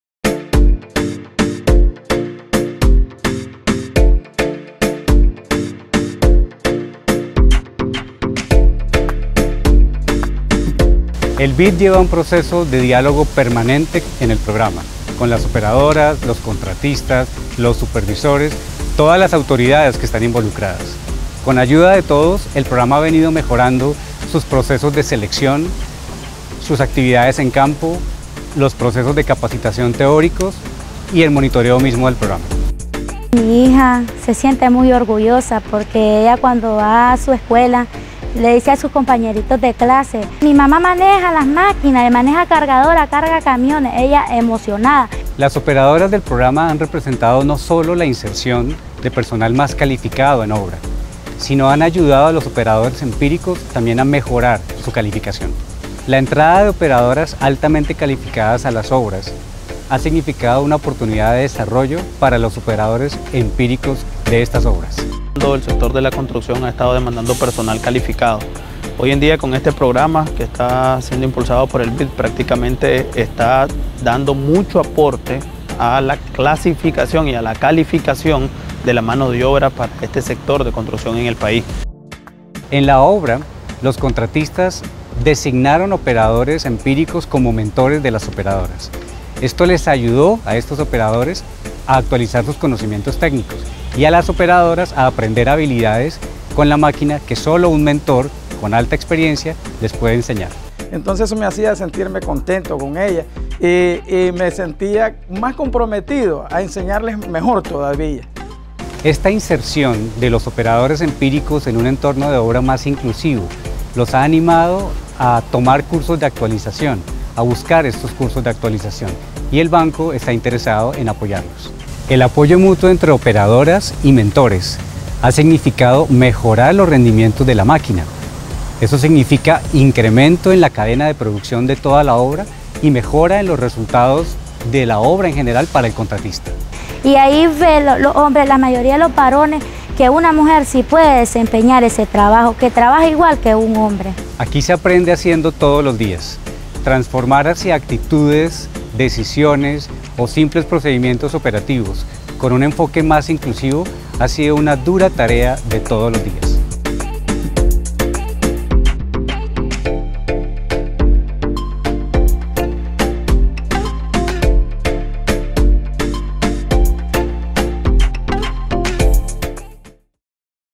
4. Programa Mujeres Operarias 2019 - Cambios en los diferentes actores
Conoce los retos y los objetivos del Programa Mujeres Operarias 2019 de Nicaragua. Así como los avances del programa y cómo está influyendo en los distintos actores de la sociedad. Acompáñanos en esta misión y ¡cambiemosle la cara al sector de la construcción!